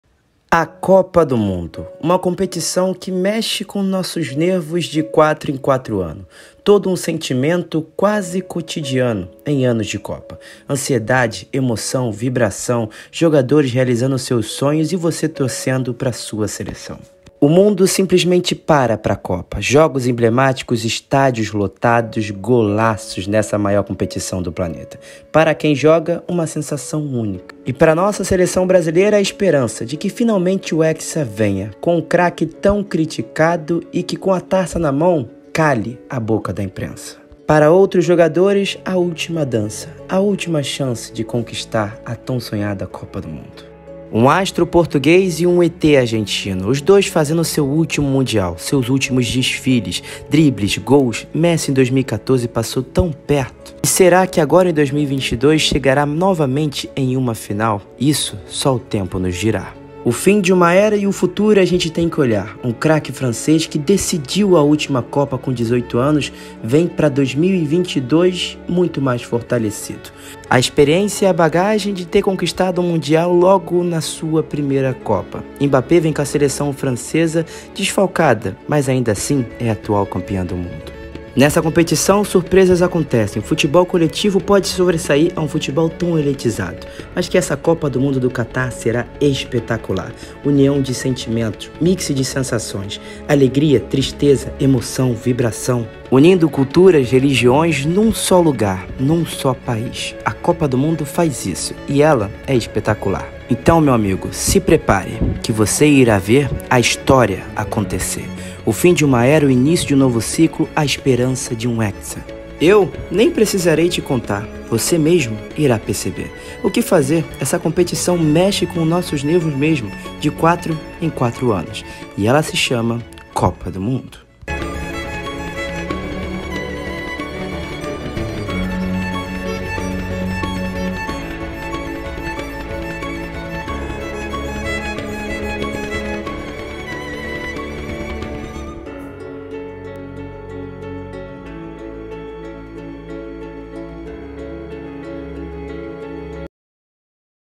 CRÔNICA DA COPA DO MUNDO 2022🏆🔥
Copa do Mundo essa competição que mexe com a gente, amantes do futebol do mundo inteiro param pra assistir a maior competição do planeta.

E não tinha como eu não fazer uma crônica sobre a Copa do Mundo de 2022 no Qatar.

Conteúdo de Qualidade🔥

Curtem, comente e Compartilhem pra todos que que amam esse esporte e principalmente a Copa do Mundo🏆⚽️